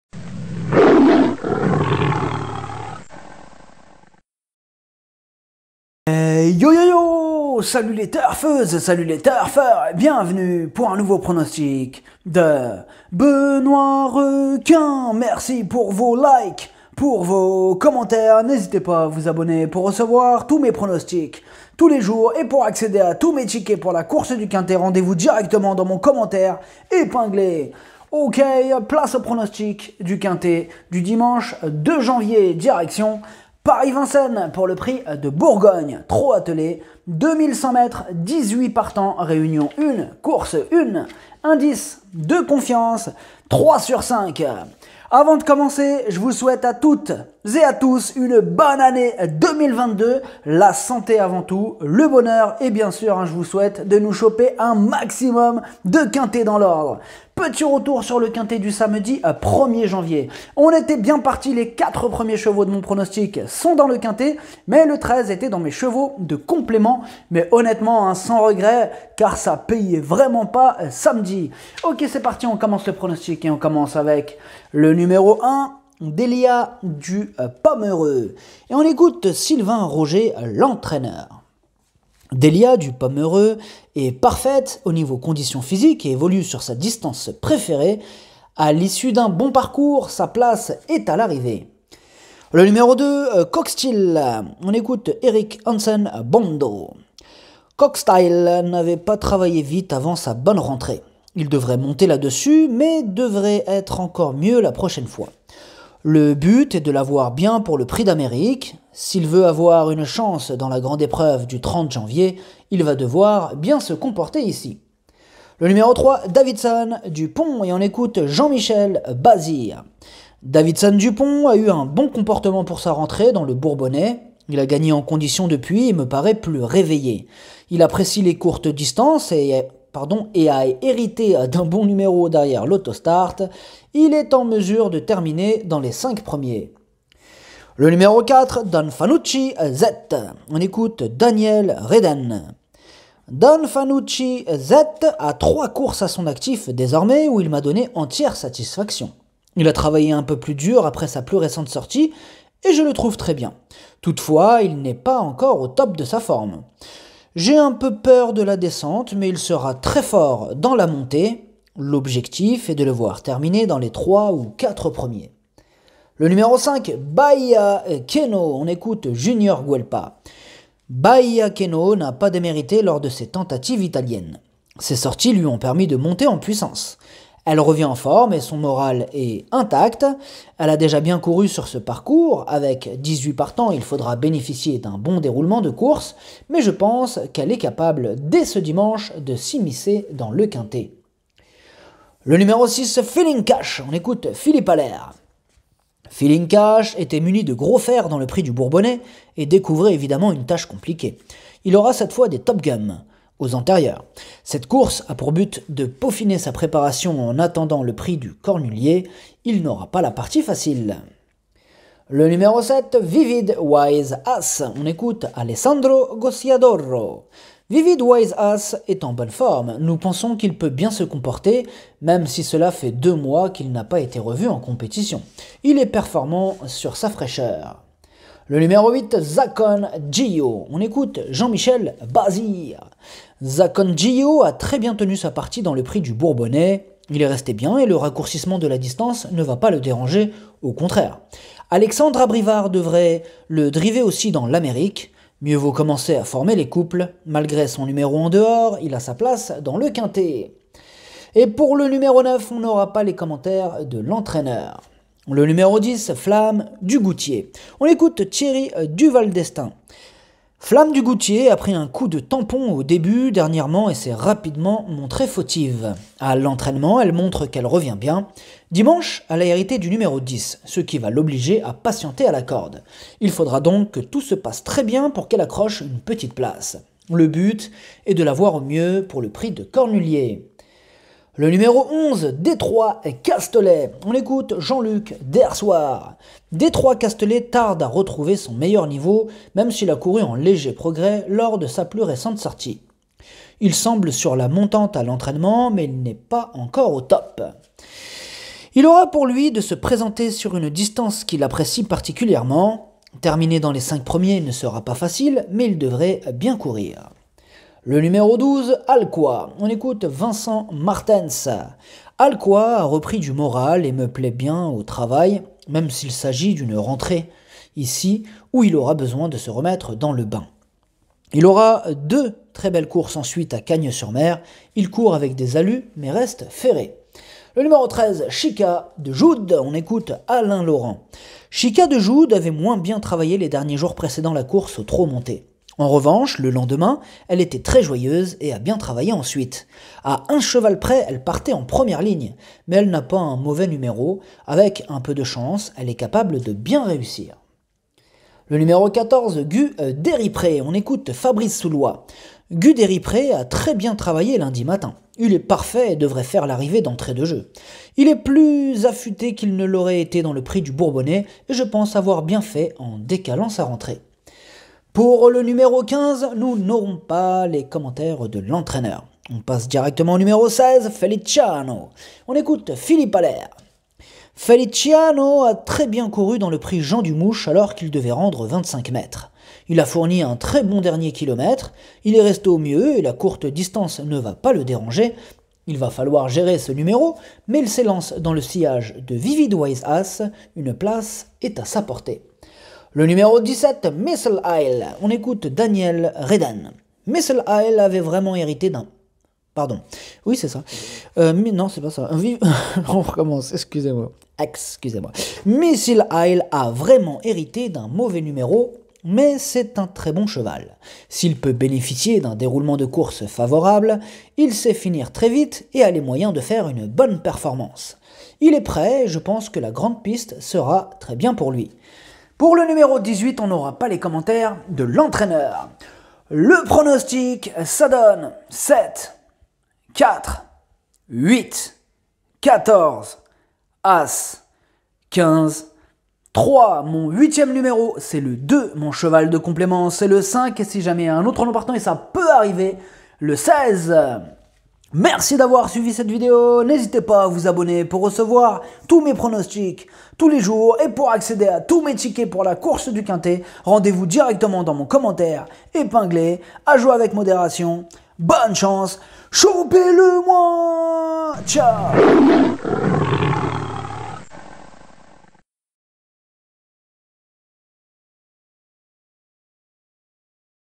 PRONOSTIC PMU QUINTE DIMANCHE 2 JANVIER 2022 QUINTE DU JOUR
PRONOSTIC PMU QUINTE DIMANCHE 2 JANVIER 2022 QUINTE DU JOUR BENOITREKIN GAGNER ARGENT DIRECTION PARIS VINCENNES ! Pour le prono et l'analyse du QUINTE du jour par BENOITREKIN pmu pour LE PRIX DE BOURGOGNE ! TROT-ATTELE, 2100 mètres, 18 partants, réunion 1 course 4 R1C4 ! Bonne chance à tous ! Gagner argent ! Choppez le moi dans l'ordre ce Quinté !!!